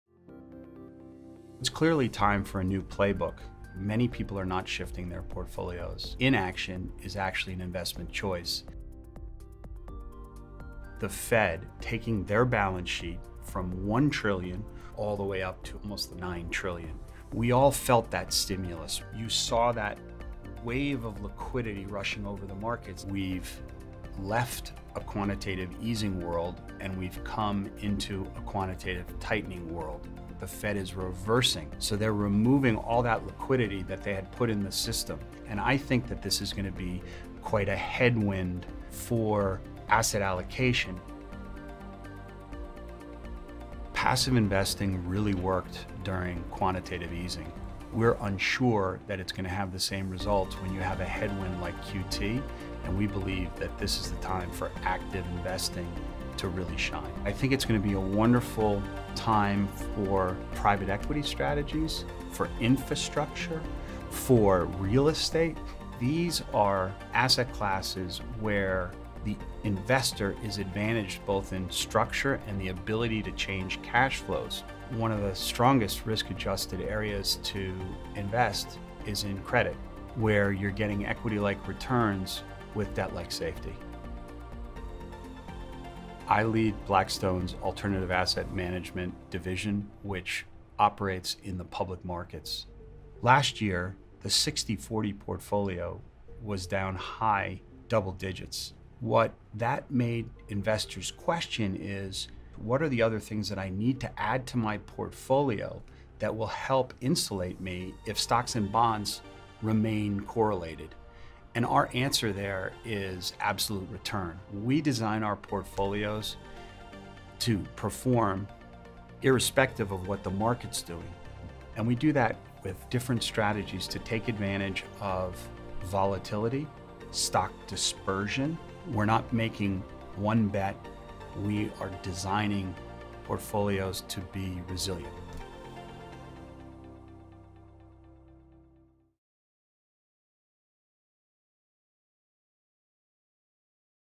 What Quantitative Tightening Means for Asset Allocation | 1x1 with Blackstone’s Joe Dowling
Despite recent strains on the U.S. banking system, the Federal Reserve has indicated that it remains committed to shrinking its balance sheet. Joe Dowling, Global Head of BAAM — Blackstone’s multi-asset platform — discusses the implications of quantitative tightening for investors and shares how his team seeks to build resilient portfolios.